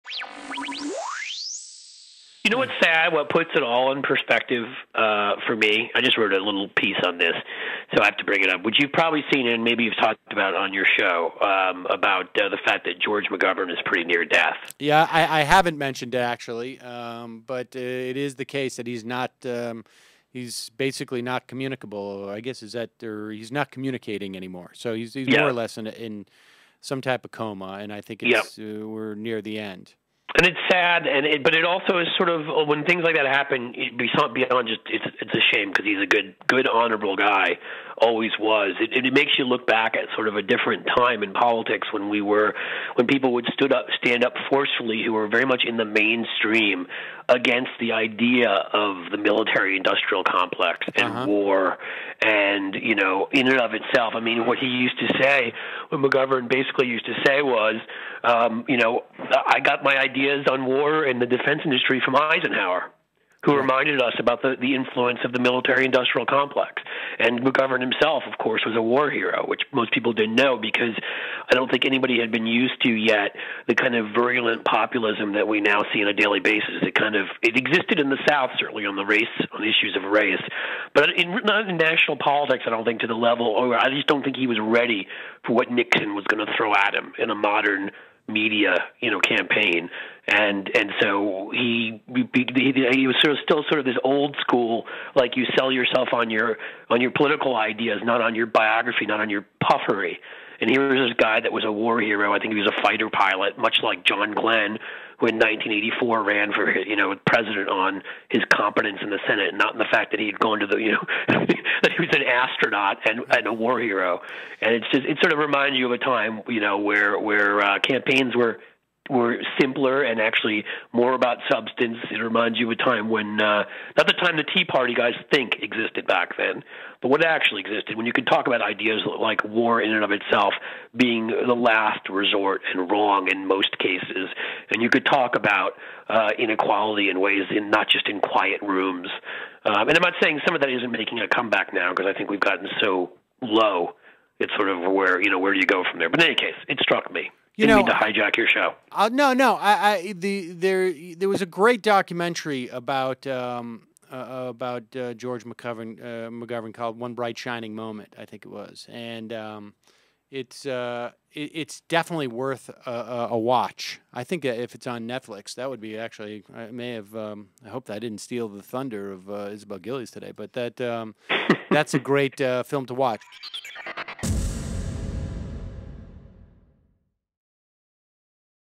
Remembering George McGovern
On Friday, before George McGovern passed away but when it was still known he was not doing well, Cliff Schecter and Sam Seder had a short look back at the former Senator and Democratic Presidential Candidate...